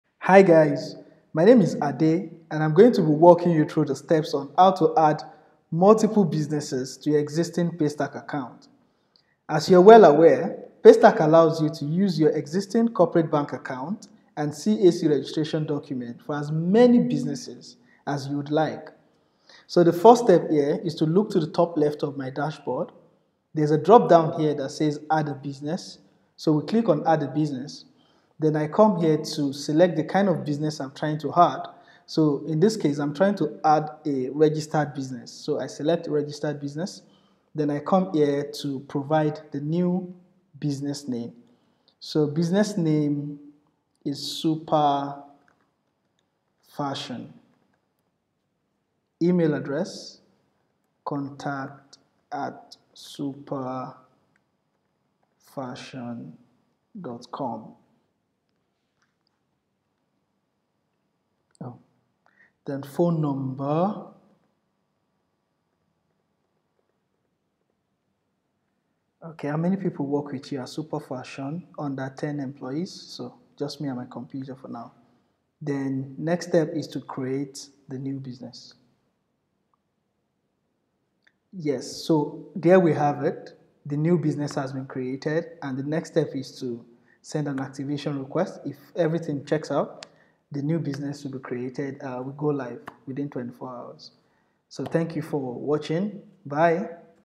How to create additional businesses from one Paystack user account
In this video, you'll learn how to create additional businesses from a single Paystack user account.

Paystack allows businesses in Nigeria to accept online payments from anyone, anywhere in the world.